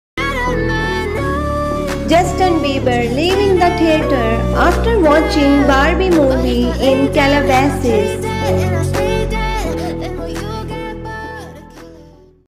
Justin Bieber leaving the theatre after watching Barbie movie in Calabasas //
Justin Bieber leaving the theatre after watching Barbie movie in Calabasas // @tcntvpk
justin bieber and hailey bieber